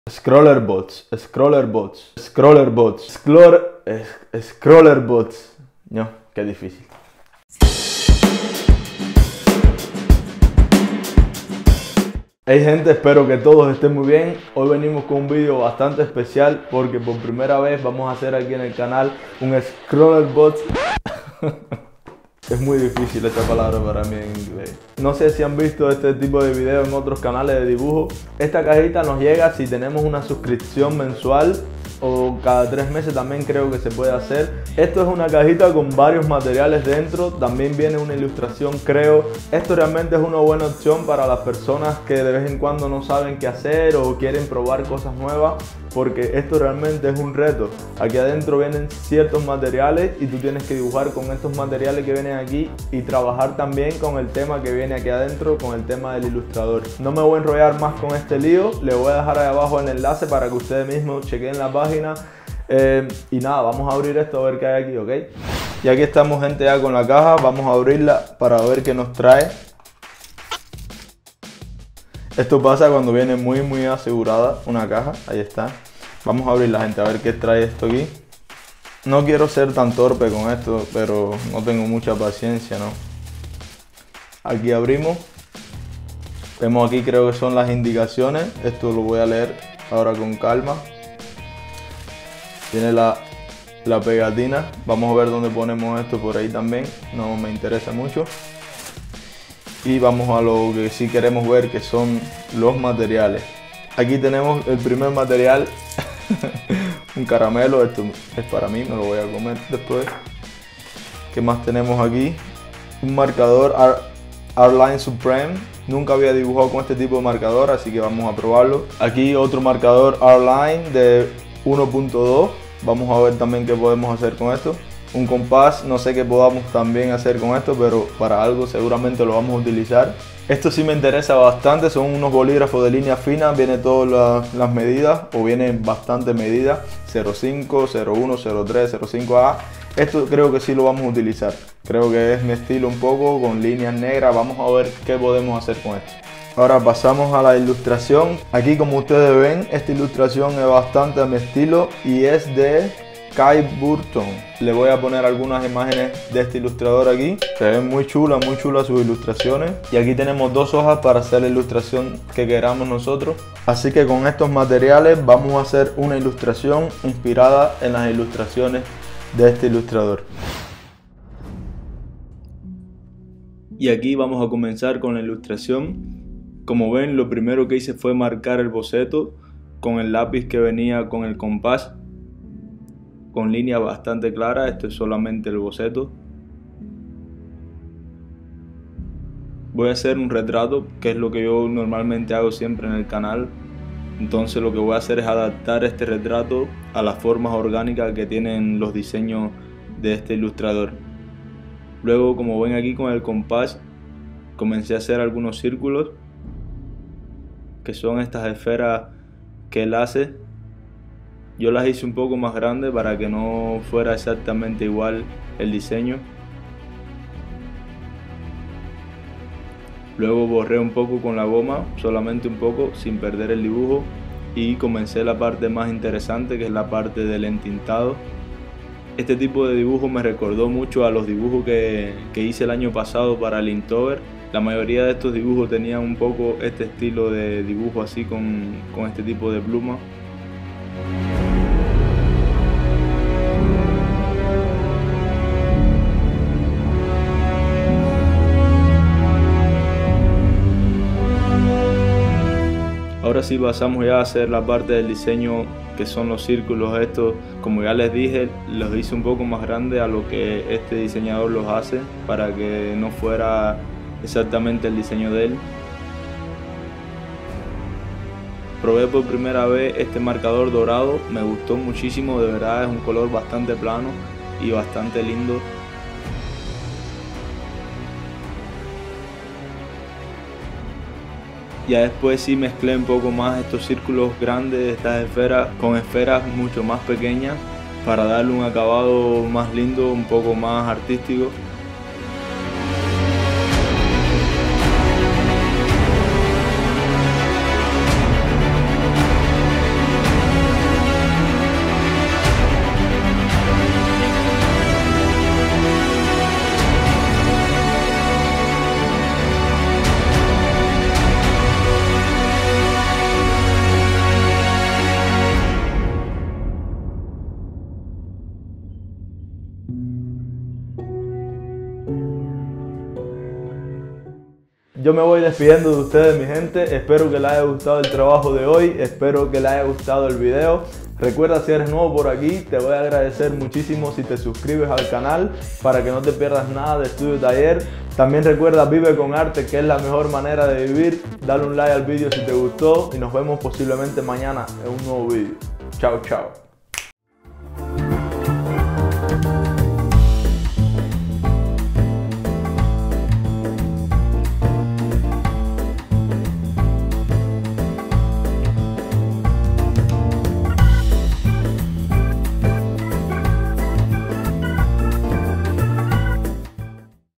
ScrawlrBox CHALLENGE Materiales de ARTE | Estudio Taller
En esta ocasión les traigo un Challenge muy divertido e interesante. ScrawlrBox CHALLENGE

Materiales que utilizo.
Carboncillo Faber-Castell
Coates - 20 carboncillos de sauce
Goma moldeable
Lápices carboncillo

Cámara
Canon G7x